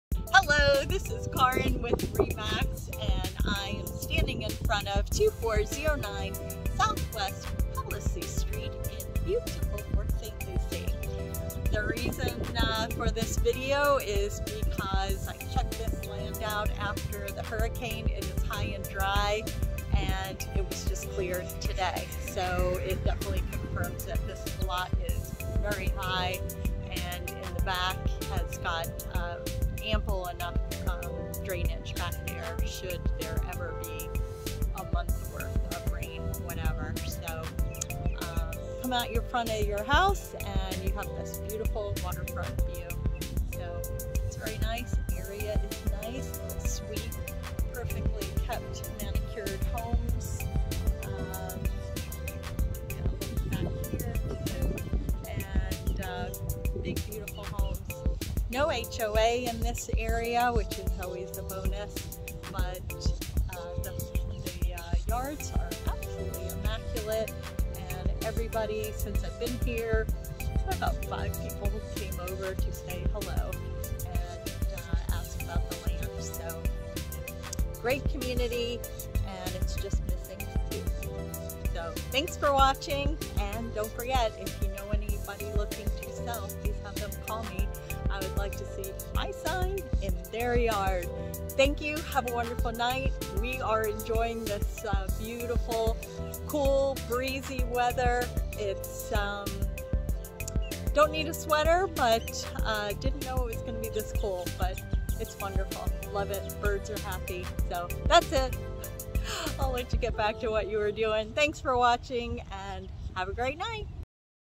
🍊4209 SW Halissee Street, Port St. Lucie🍊Improvements completed, ☎️ Call me 1st, then your Builder!
🍊4209 SW Halissee Street, Port St. Lucie🍊Improvements completed, Land is Cleared, High & Dry! Call Me 1st ☎️ Then your Builder!